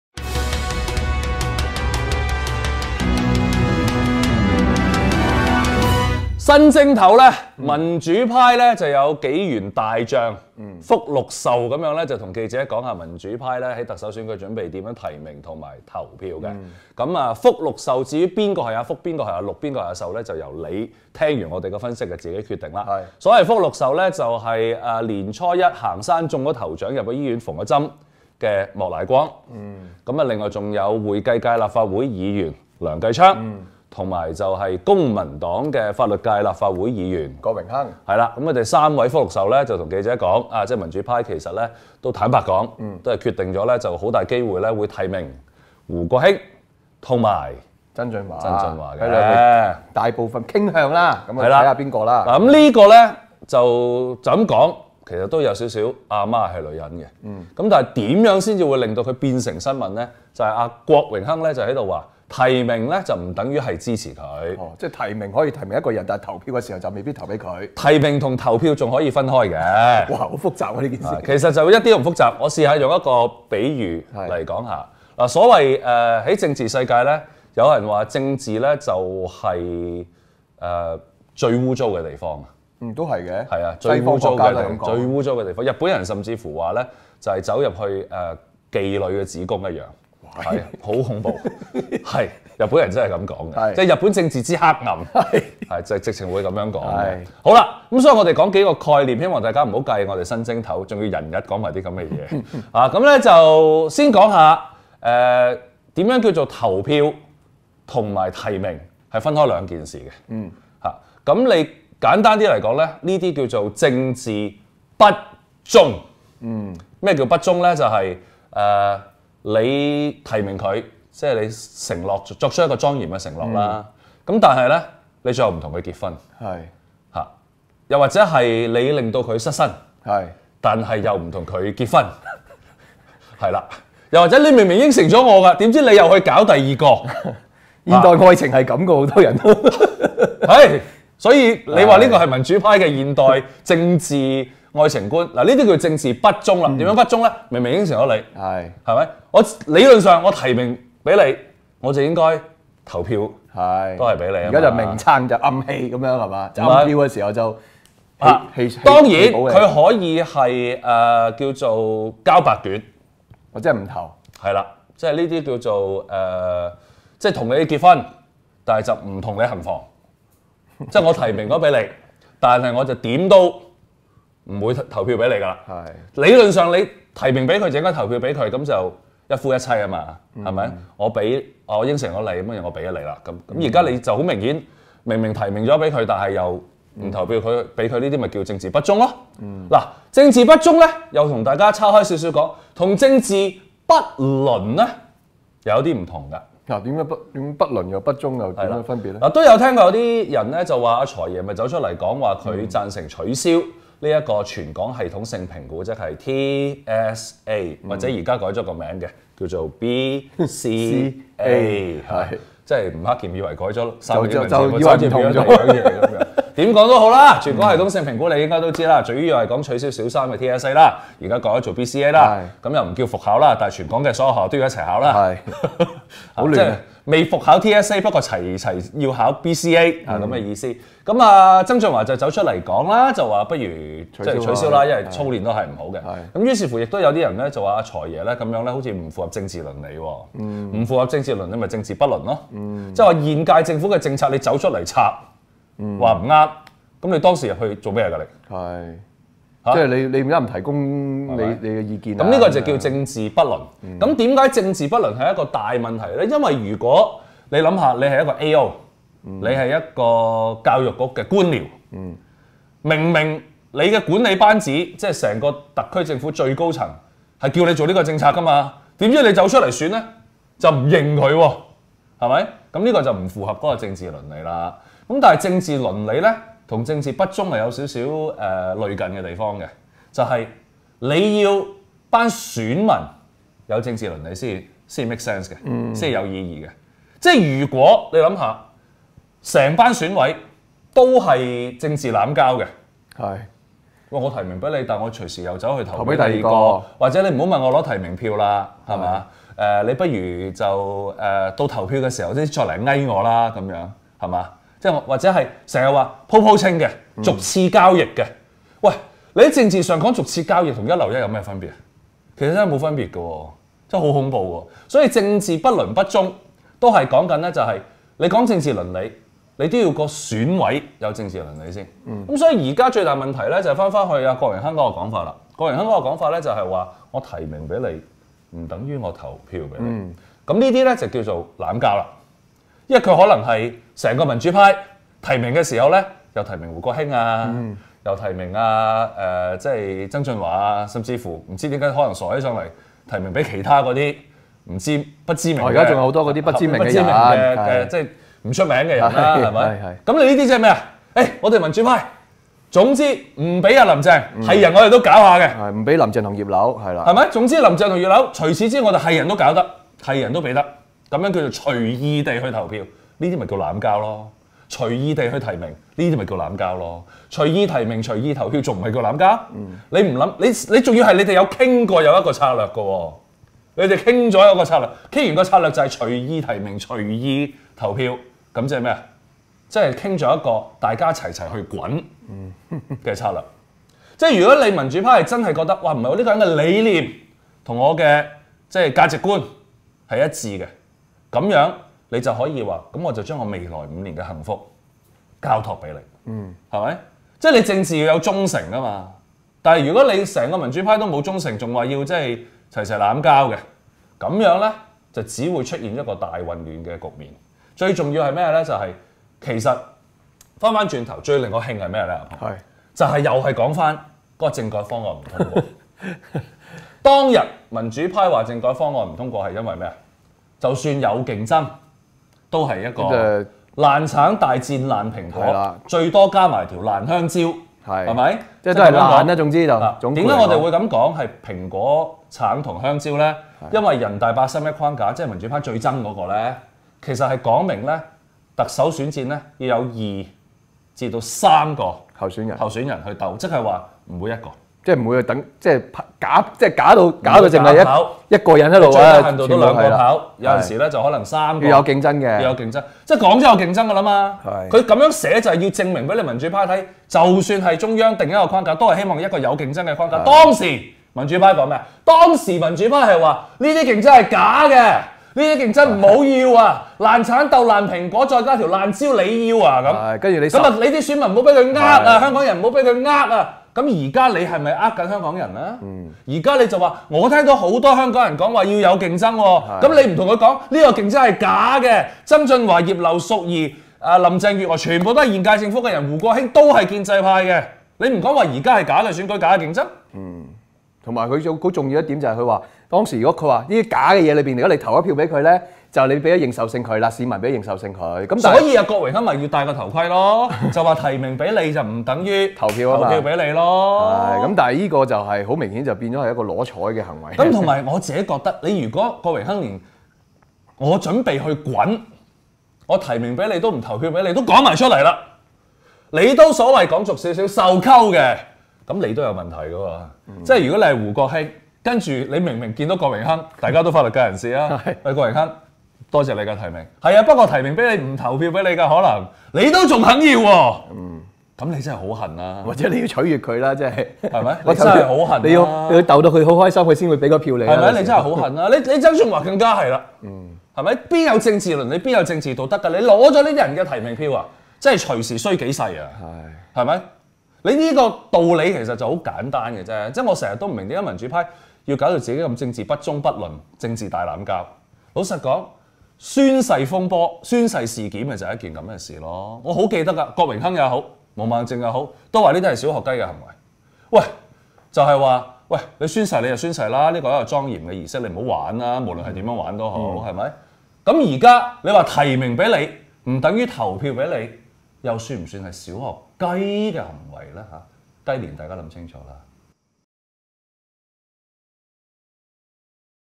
解困新聞：郭榮鏗叫民主派齊齊政治濫交
郭榮鏗昨日表示民主派傾向提名曾俊華和胡國興參選特首。問及有候選人曾表示應在831框架下實行政改，提名是否違背泛民理念？郭卻回應指提名不等於支持。如此一來，則陷泛民於不忠窘境，因提名理論上即等於支持，如今隨意提名和投票，可算是政治濫交？當年政改啟動，泛民堅稱即使有普選，都只是㨂爛橙、是假競爭，為何今次又會突然轉軚？